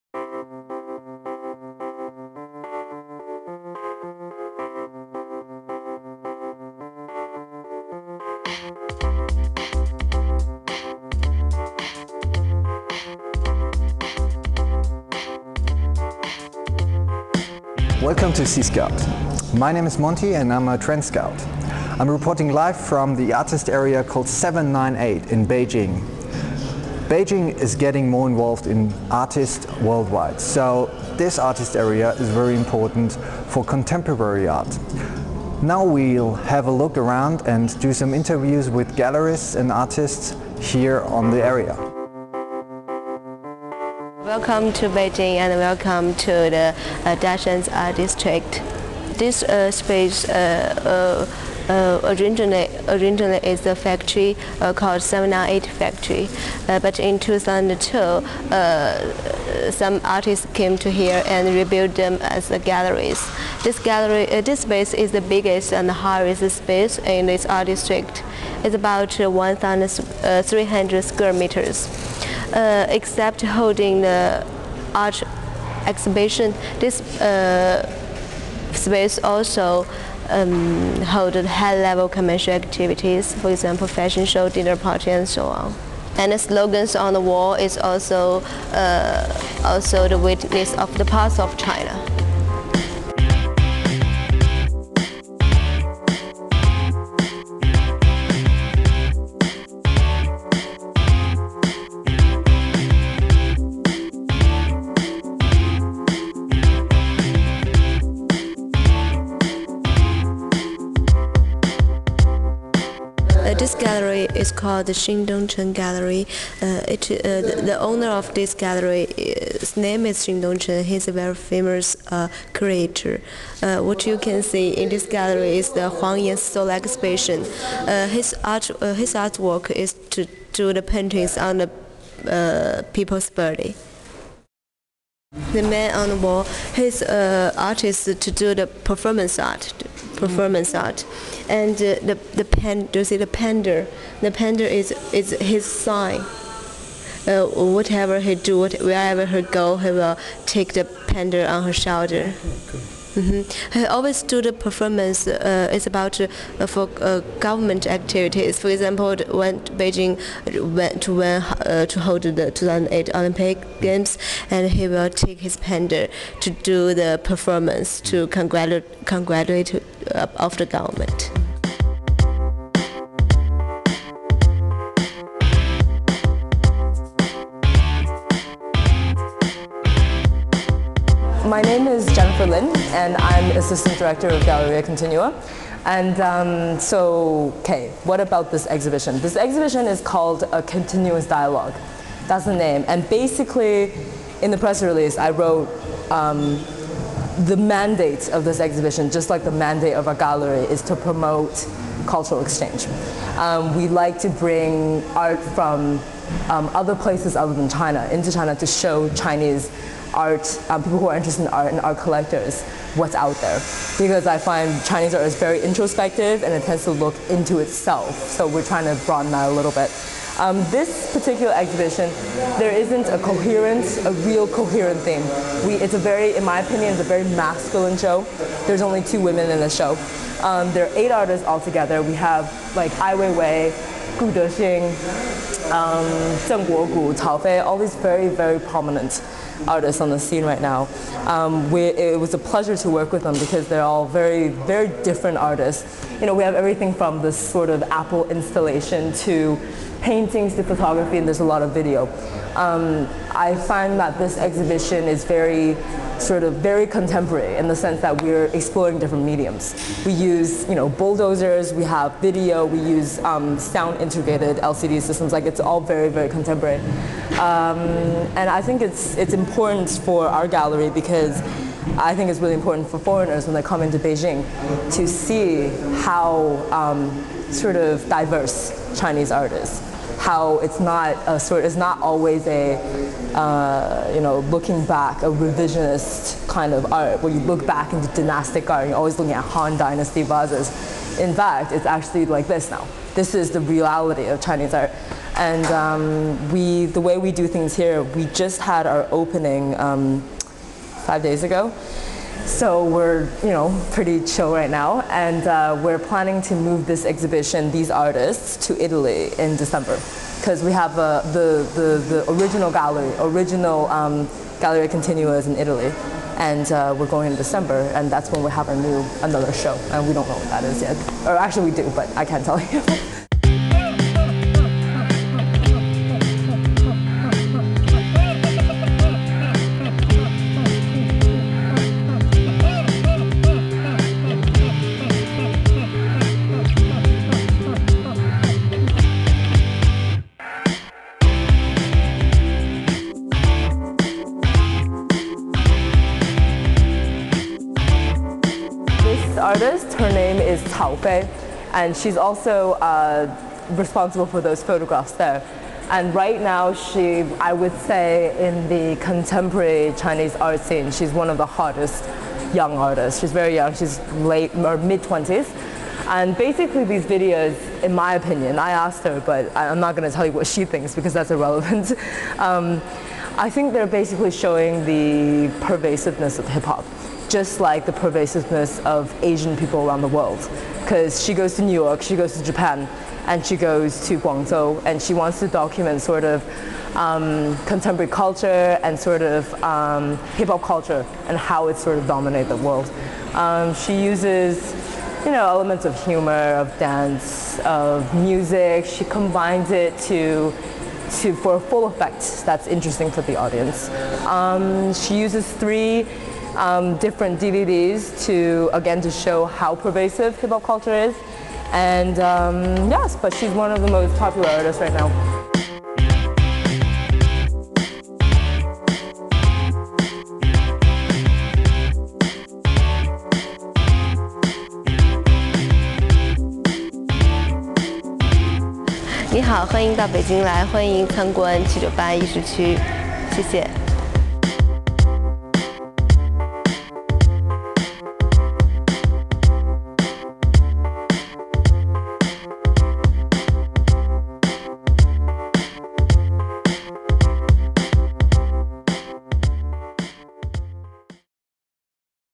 Global Trend Scouting -Artist Area 798
CScout is a global Trend Consultancy with offices in New York, Munich, Beijing and Tokyo.
During a recent TrendTour we visited the Artist Area "798" in Beijing, China. Beijing is preparing for the Olympic Games in 2008 and our local Team is reporting about the newest developments to keep our customers on the pulse.
This is the TrendTour Documentation Video with Interviews and Backgrounds about this creative area.

Produced & Filmed by Monty Metzger
for CScout Trend Consultancy and manager tours